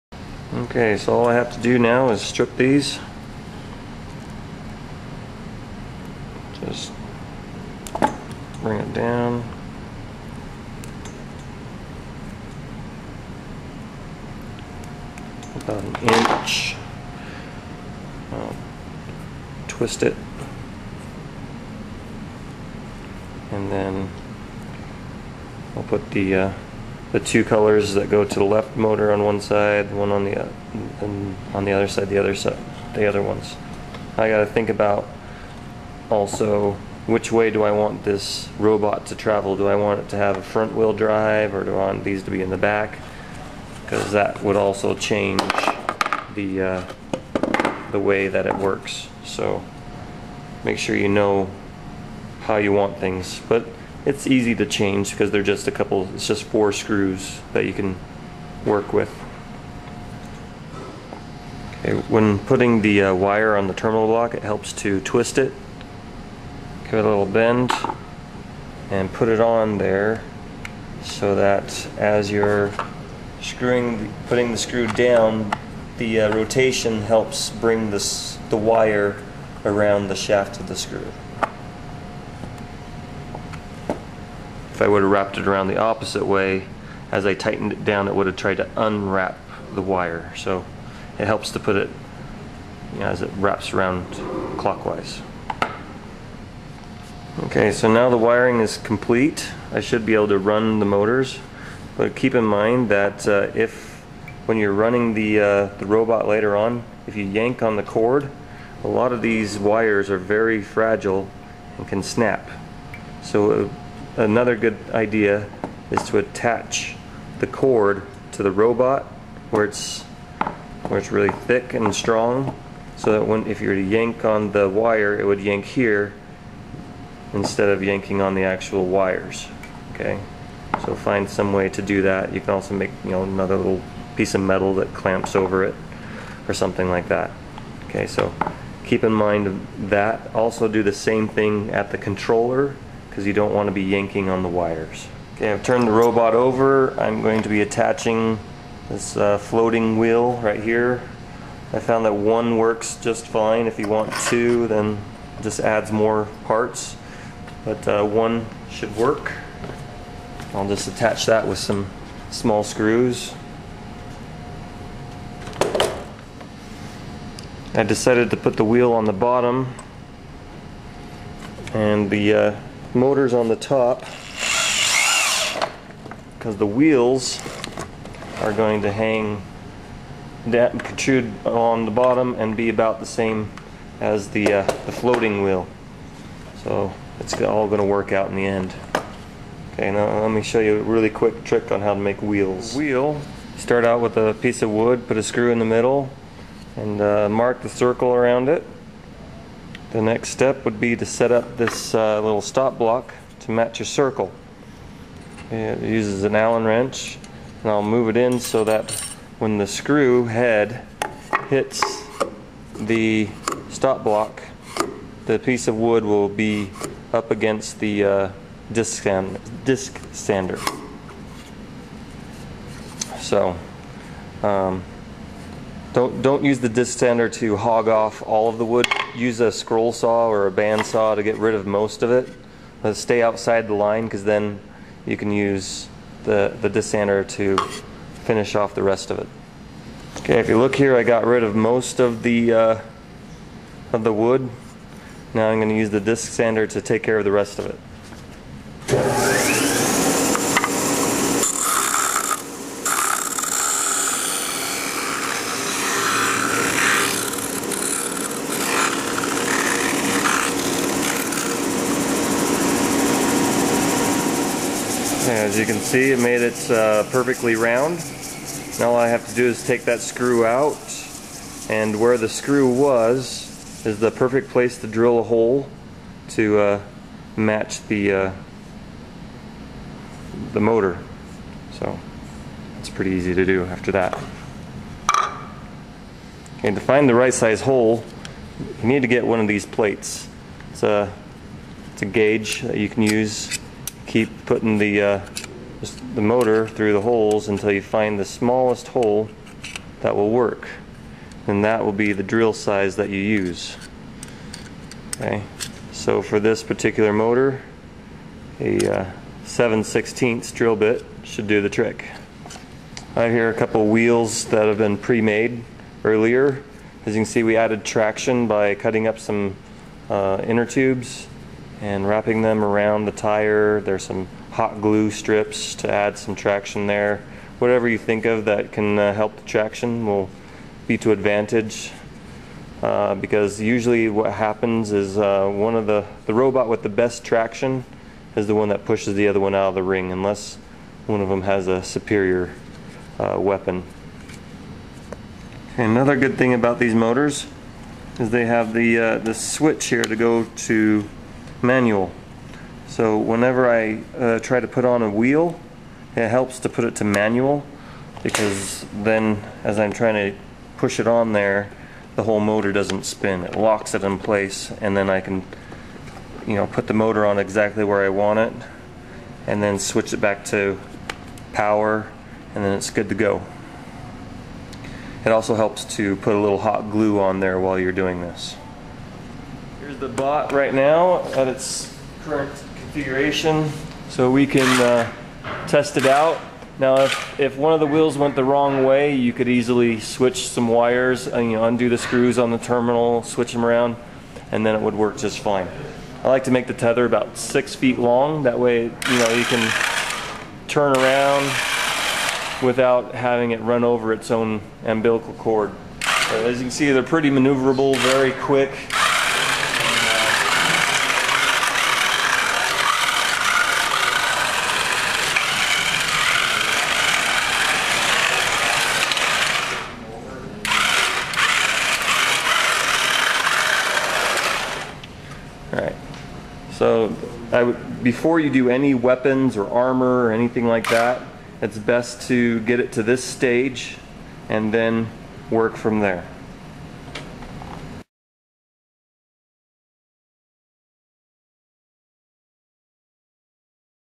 how to make a battle bot part 4
These are small battle bots for a beginning engineering class at Wasatch junior high.